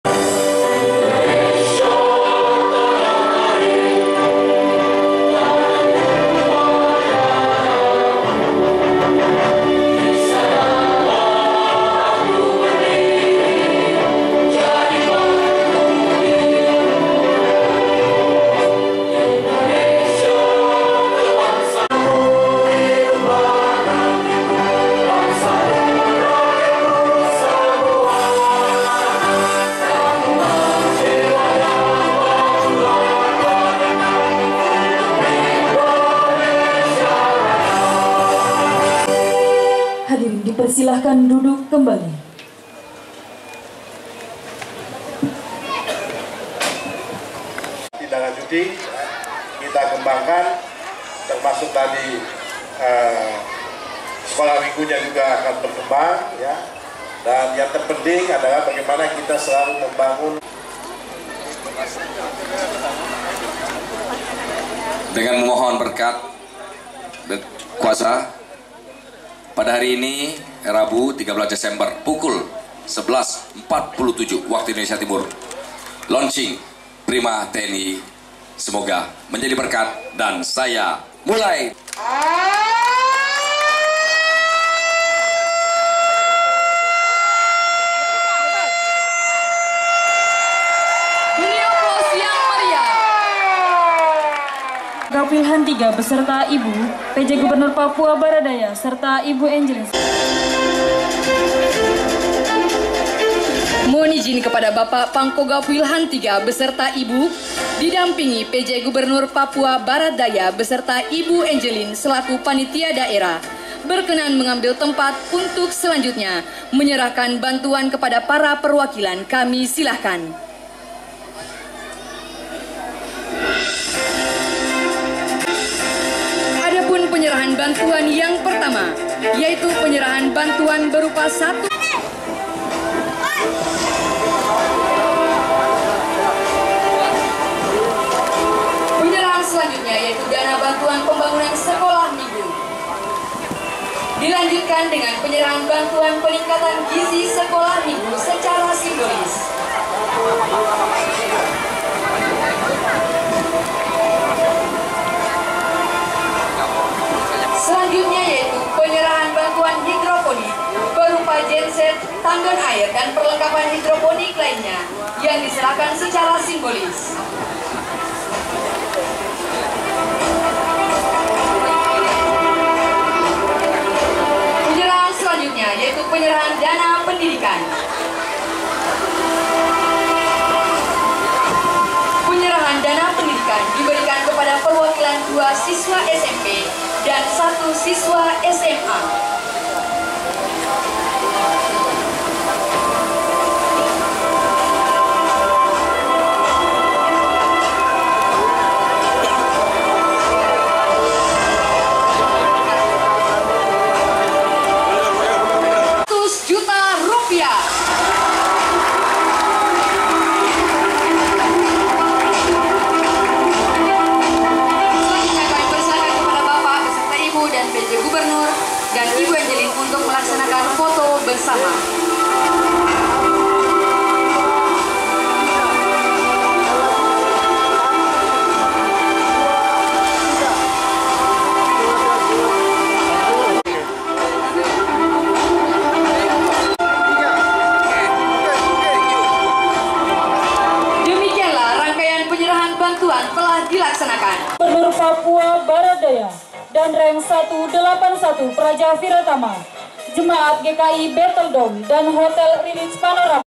Detik-detik Lounching Program Prima TNI Di GKI Bethel Doom Sorong Papua Barat Daya
Letjend TNI Richard Tampubolon Bersama Pj Gubernur dan Pendeta Lounching Program Prima TNI Di GKI Jemaat Bethel Doom Sorong Papua Barat Daya Tahun 2023